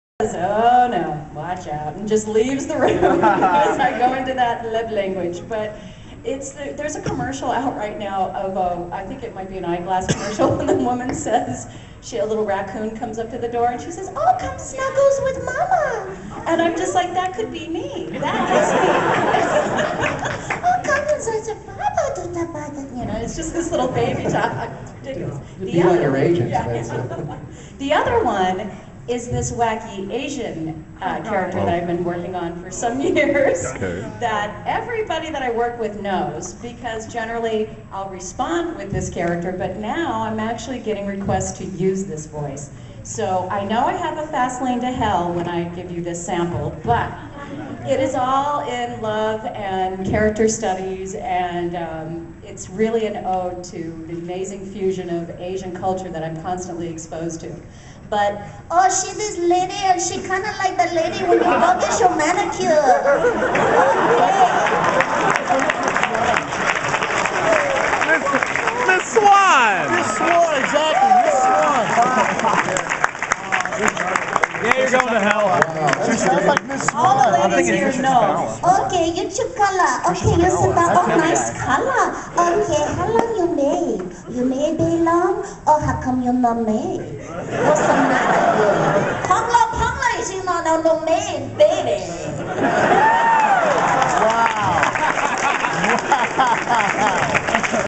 AX 2010 - Wendee Lee's Unused Voices
Someone in the audience asked, "Are there any voices you haven't used or want to use in an anime?"

Wendee Lee then demonstrated two of her unused voices: a baby-talk voice (which she showed just before the start of this video, but she shows off again near the beginning of the clip) and then a stereotypical Asian voice.  Listen close and you'll be able to hear my take on who she sounds like... =)